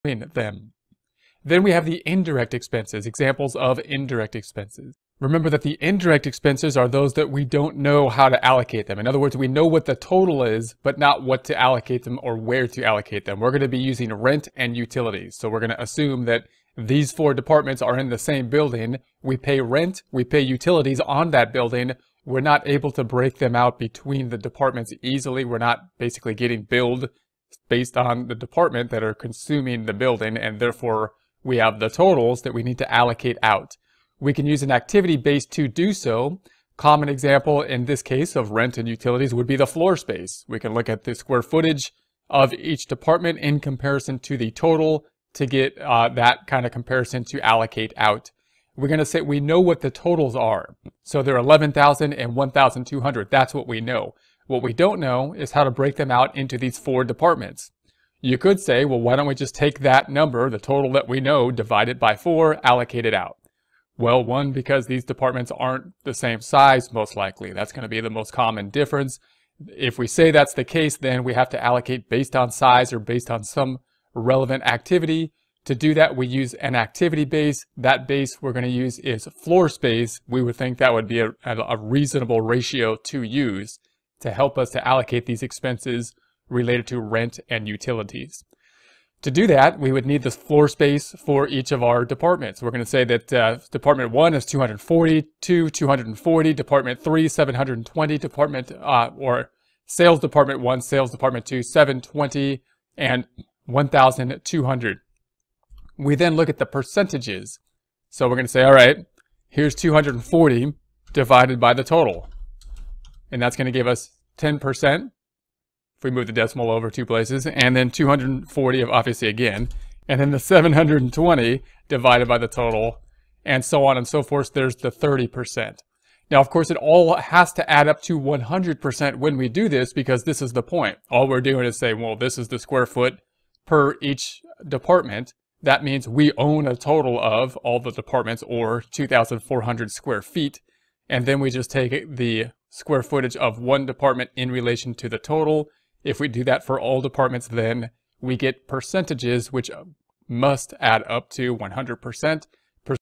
Steps to Create the Departmental Income Statement Responsibility Accounting
Learn the essential steps to create a departmental income statement in the context of responsibility accounting. This video breaks down each step clearly, helping you understand how to accurately compile and allocate revenues and expenses across departments.